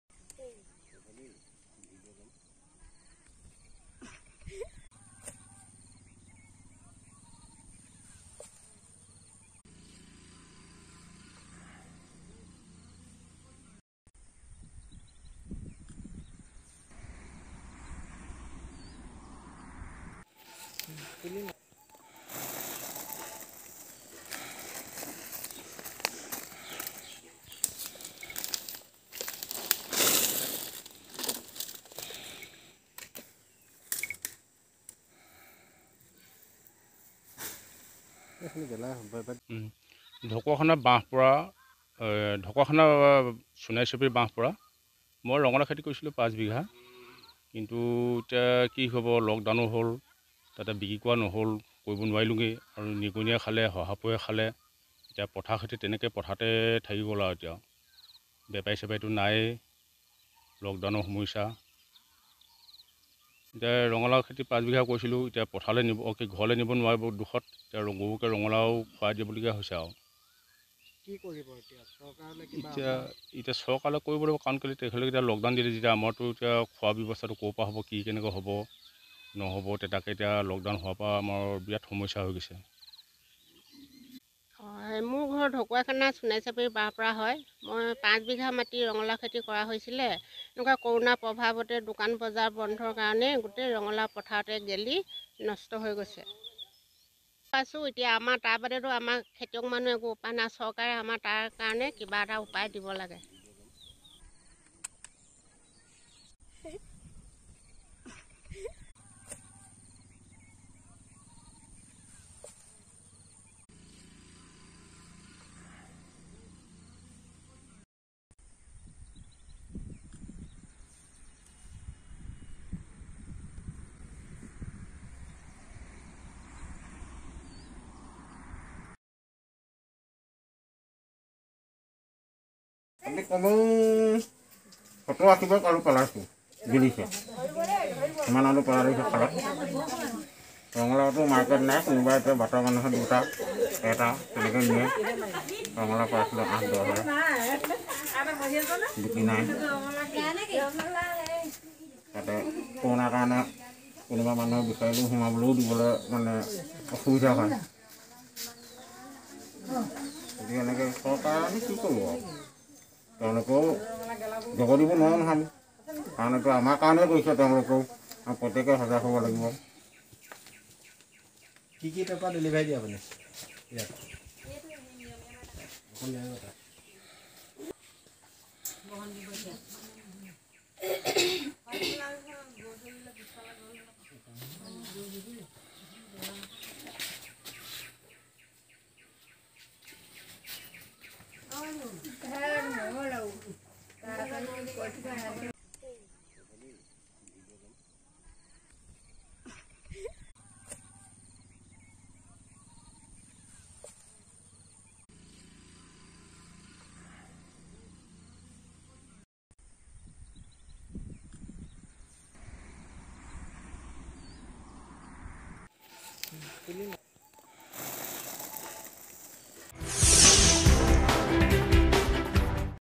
কৃষকৰ জীৱনলৈ নামি আহিছে কাল অমানিশা
কৃষকৰ জীৱনলৈ নামি আহিছে কাল অমানিশা, গেলিব ধৰিছে কৃষকৰ উৎপাদিত কুইন্টলে কুইন্টলে আলু আৰু ৰঙালাও, ঢকুৱাখনাত বজাৰৰ অভাৱত লোকচানৰ সন্মুখীন হৈছে কৃষক